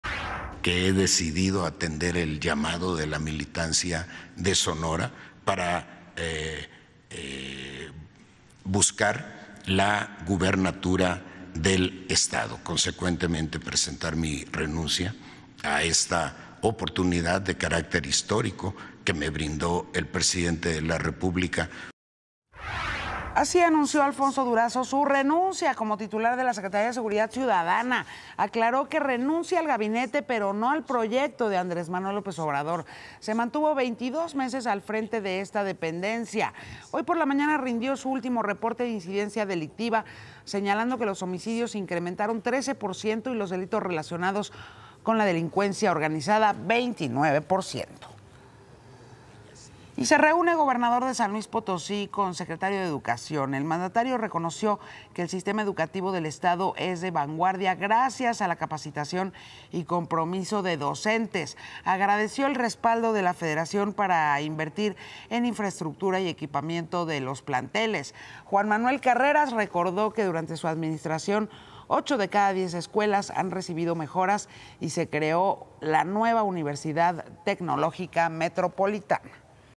Durazo deja la Secretaría de Seguridad y va por la gubernatura de Sonora | Noticias con Yuri Sierra
Así anunció AlfonsoDurazo su renuncia como titular de la Secretaría de Seguridad y Protección Ciudadana para ir por la gubernatura de Sonora.